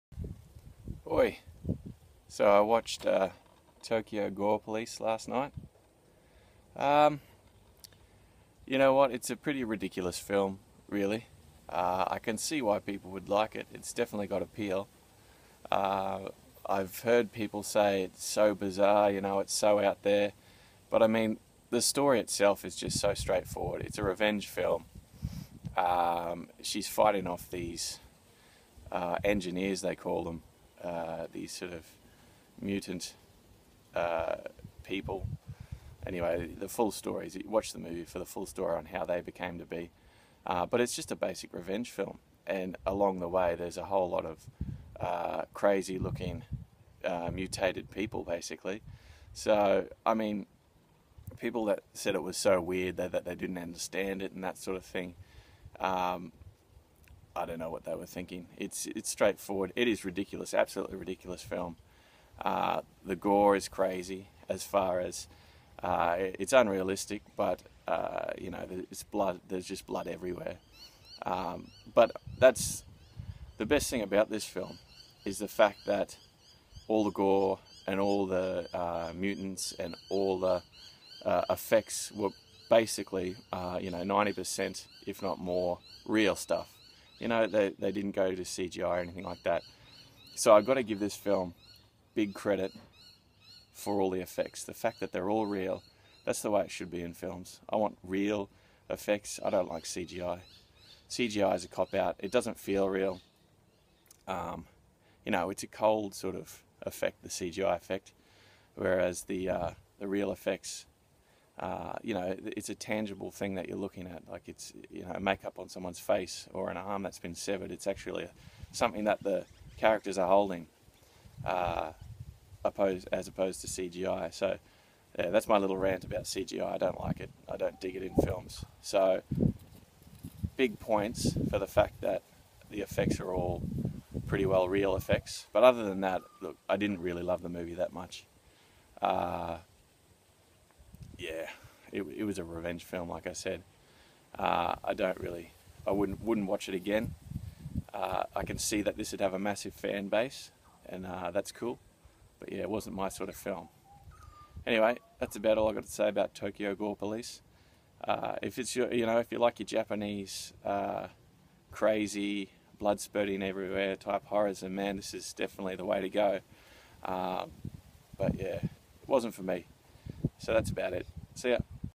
Movies 11: TOKYO GORE POLICE (2008)
Totally ridiculous ... I can see its appeal ... well done as far as the REAL blood, guts & gore effects go!